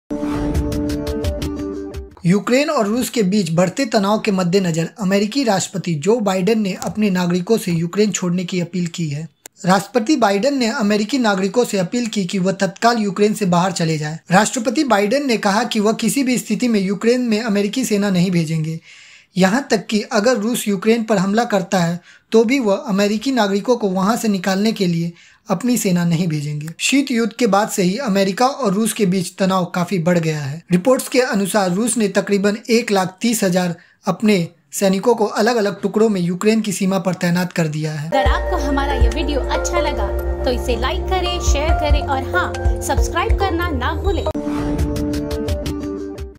राष्ट्रपति बाइडेन ने अमेरिकी नागरिकों से यूक्रेन छोड़ने की अपील की
यूक्रेन में बढ़ते तनाव को देखते हुए अमेरिका के राष्ट्रपति जो बाइडेन ने अमेरिकी नागरिकों को यूक्रेन छोड़ने के लिए कहा है। राष्ट्रपति बाइडेन ने अमेरिकी नागरिकों से अपील की है कि वह तत्काल यूक्रेन से बाहर चले जाएं। राष्ट्रपति बाइडेन ने अमेरिकी नागरिकों से कहा कि वह किसी भी स्थिति में यूक्रेन में अमेरिकी सेना नहीं भेजेंगे, यहां तक कि अगर रूस यूक्रेन पर हमला करता है तो वह अमेरिकी नागरिकों को वहां से निकालने के लिए भी अपनी सेना नहीं भेजेंगे। बता दें कि शीत युद्ध के बाद से अमेरिका और रूस के बीच तनाव काफी ज्यादा बढ़ गया है। रिपोर्ट के अनुसार रूस के तकरीबन 130000 सैनिक अलग-अलग गुट में यूक्रेन की सीमा पर जमा हैं।